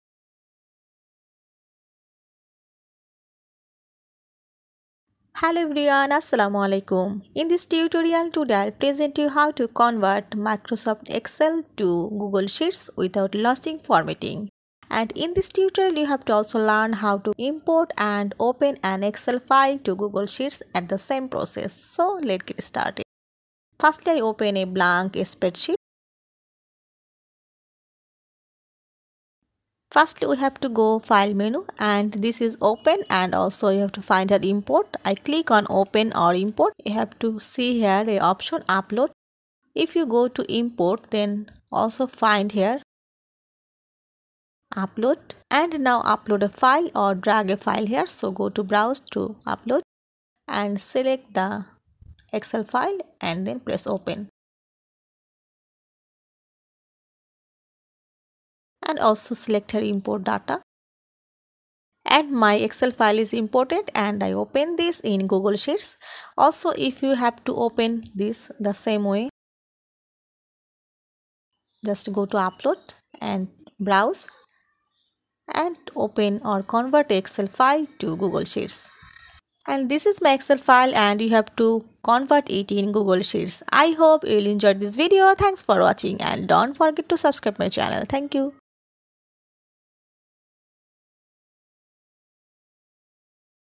How to convert Microsoft Excel to Google Sheets without losing formatting
Hello everyone, Today I will present you, How to convert Microsoft Excel to Google Sheets without losing formatting.